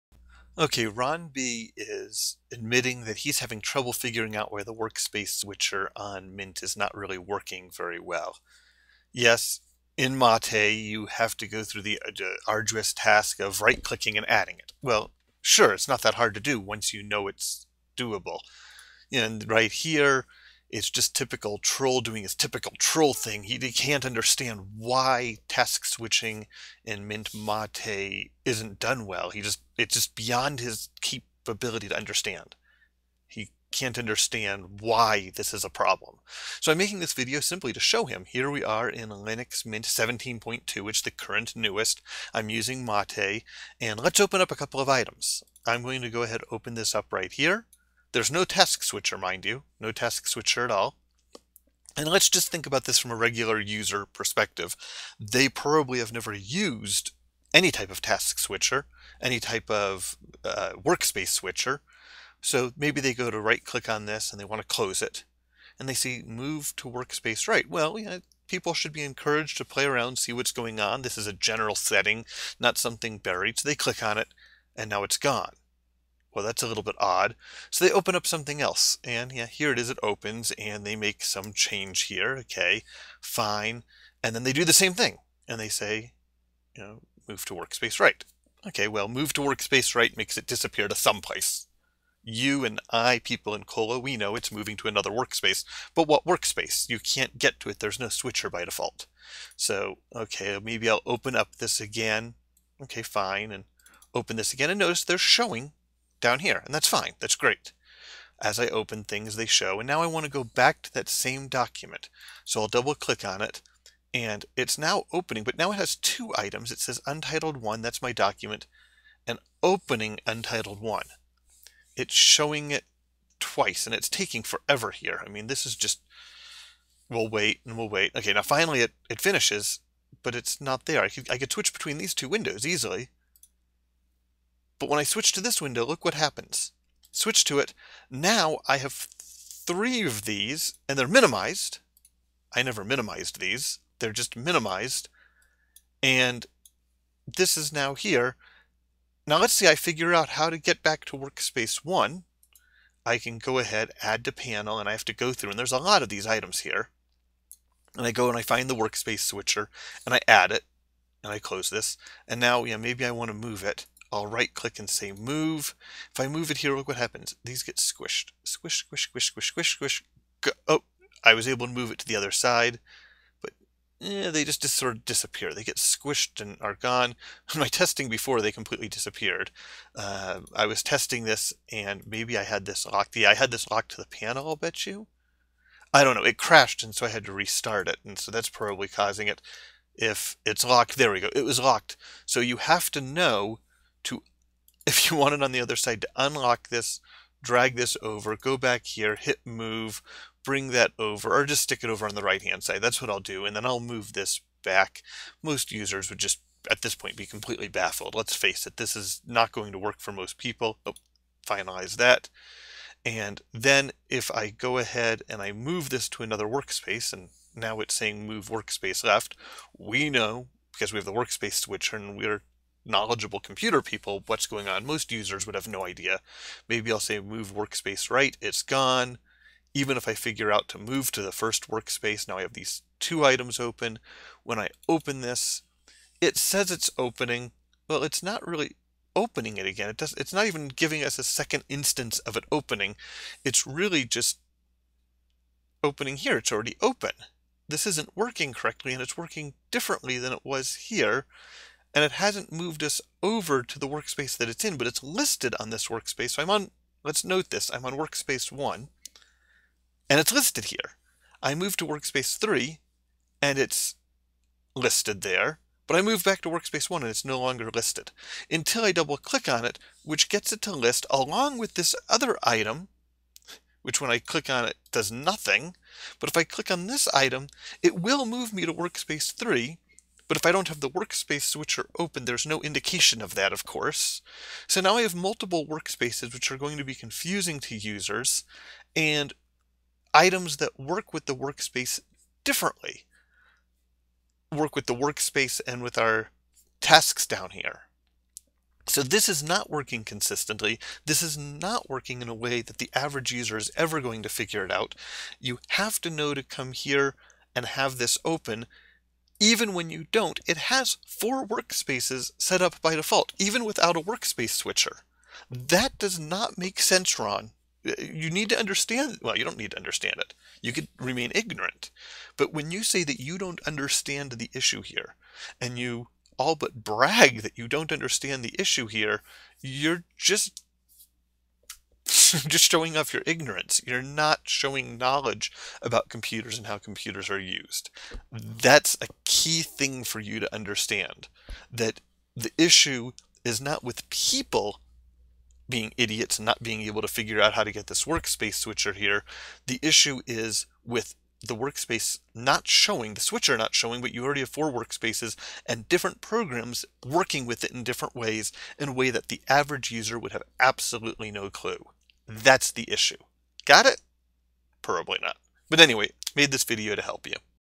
Mint Mate 17.2 Workspace Switcher
Workspace Switcher in Mint Mate 17.2 and its oddities.